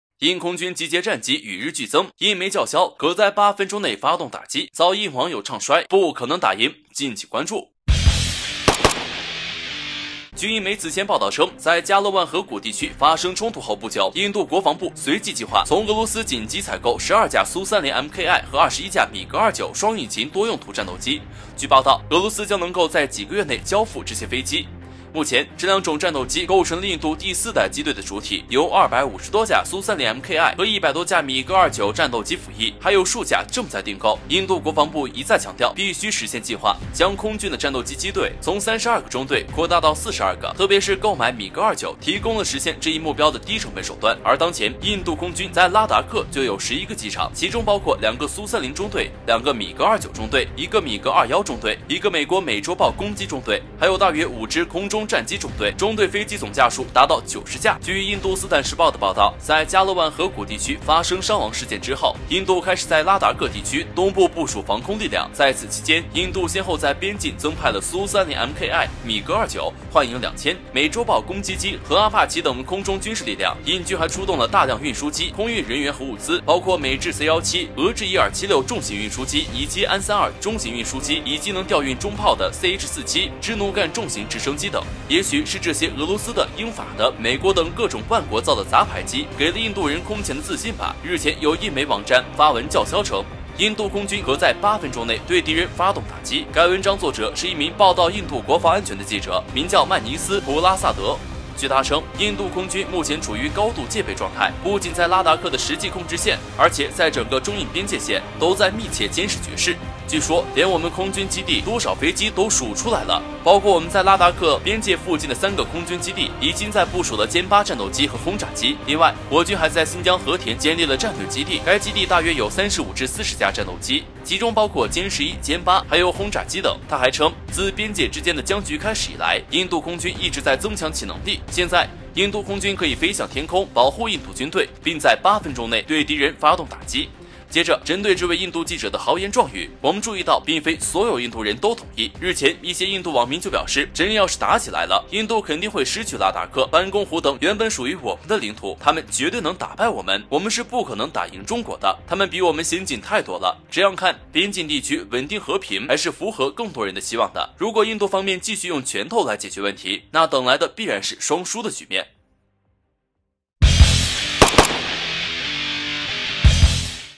印空軍大批戰機邊境集結，可8分鐘內完成對敵打擊？印網民：不可能打贏中國 【一号哨所】
也许是这些俄罗斯的、英法的、美国等各种万国造的“杂牌机”给了印度人空前的自信吧，日前有印媒网站发文叫嚣称，“印度空军可在8分钟内对敌人发动打击。 我們的影片會遵循公平、尊重的原則，確保不會包含任何仇恨言論的元素。